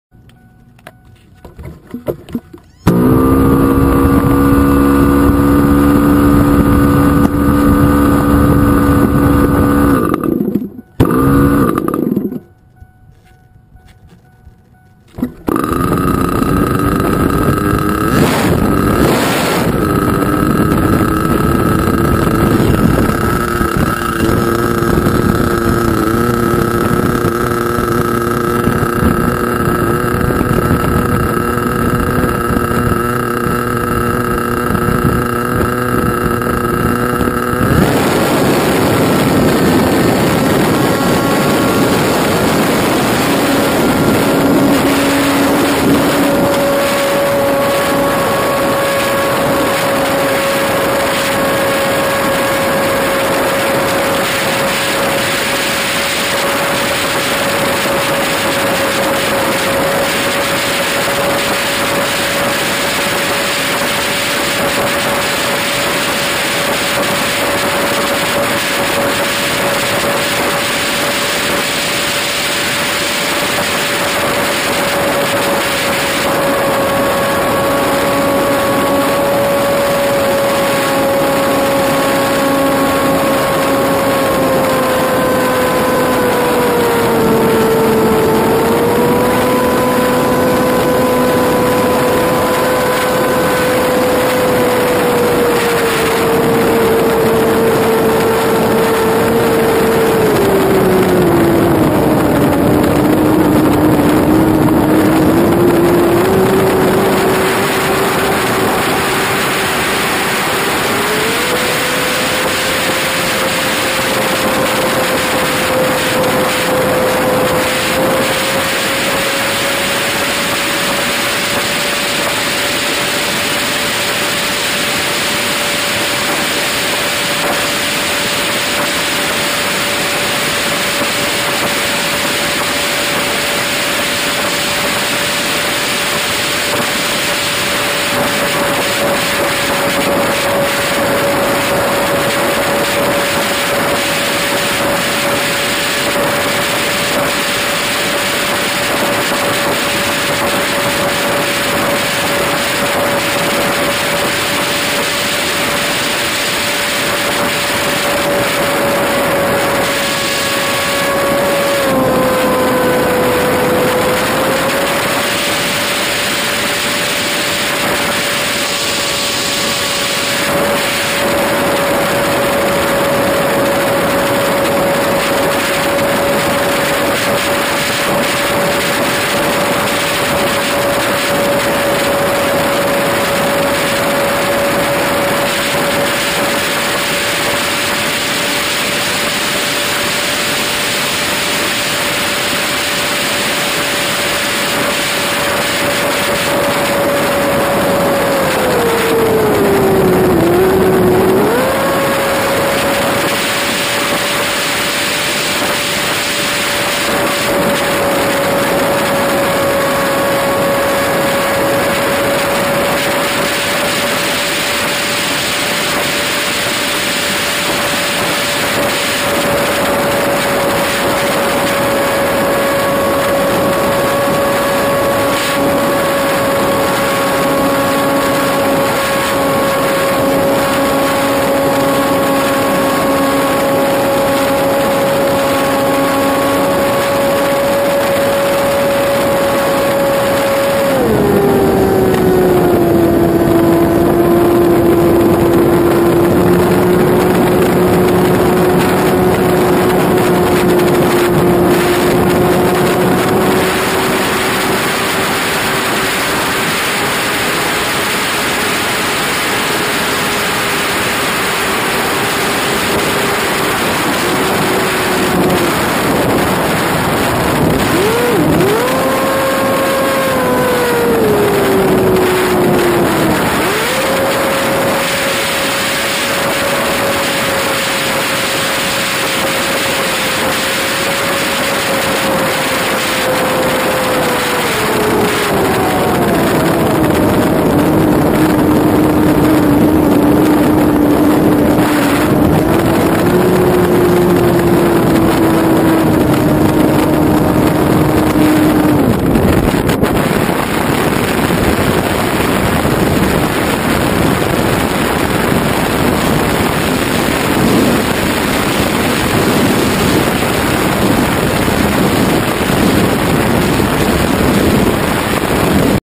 quickie 500 onboard gopro
rossi .45 modified shaved lightened crankshaft plus ported crank airway and piston sleeve.smoothed up exaust manifold